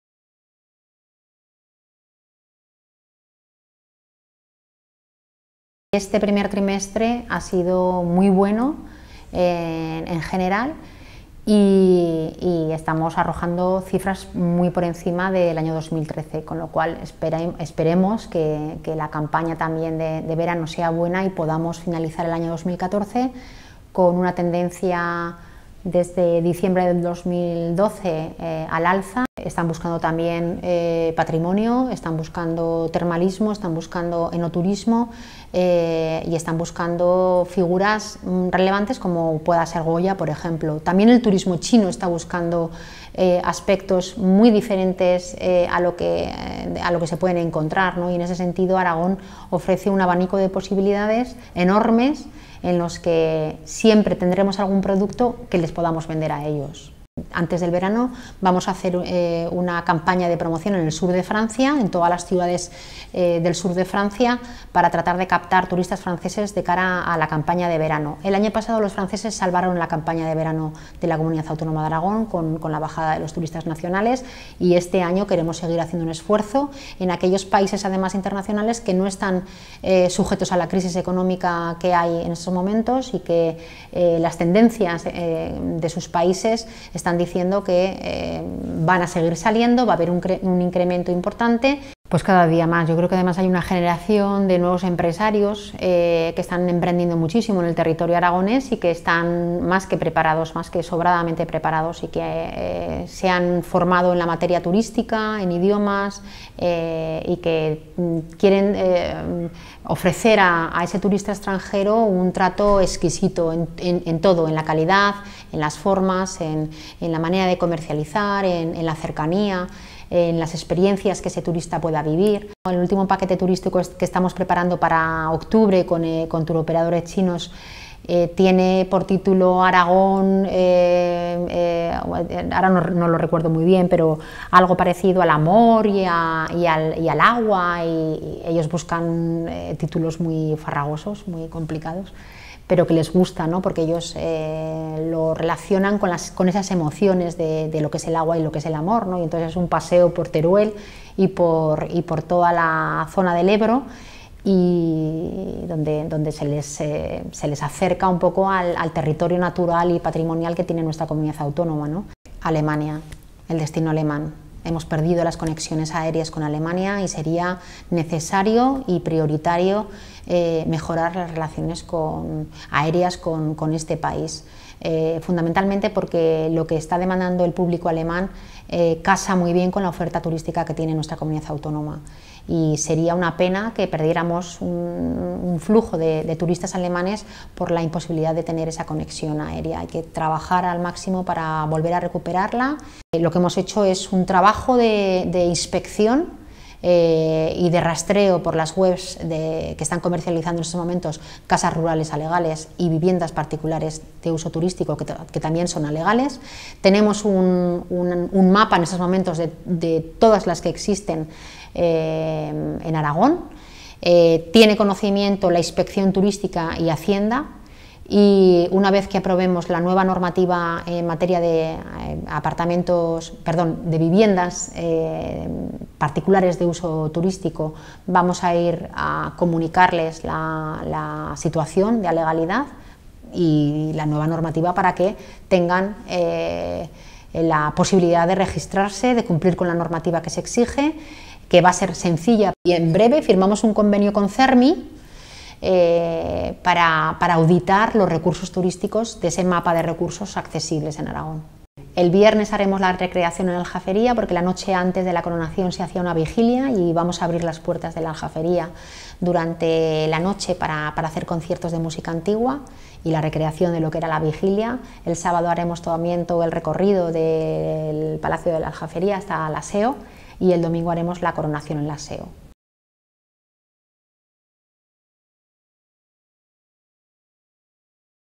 "Las inspecciones han detectado unos 500 alojamientos ilegales que se están comercializando"
(26/05/2014) La directora general de Turismo, Elena Allué, tiene ante sí el reto de impulsar el turismo responsable. Allué da a conocer en esta entrevista que en la Comunidad hay unos 500 alojamientos turísticos ilegales a los que se les comunicará su situación tras aprobar la nueva normativa, que estará lista después del verano.